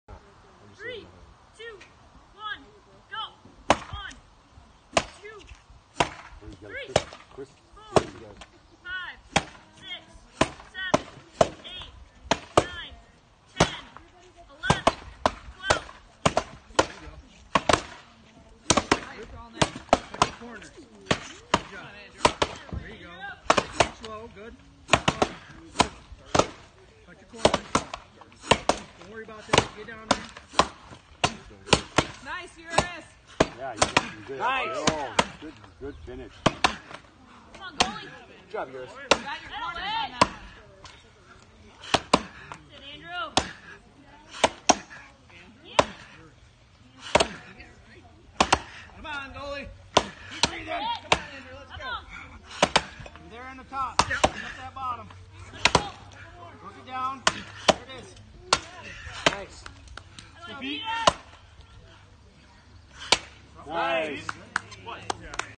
Men's 2nd chance underhand handicap. Shawano alternate 2024. Big PR for me getting ~31 sec.
I was given the handicap to start on GO due to my poor performance in the first heat. BUT being warmed up by now and using a different axe that liked the wood MUCH better. I was able to buy up a chopping performance that was significantly better. Underhand PR for me at 31 seconds. missed my goal by 2 seconds. I really want to break the 30 second mark this year.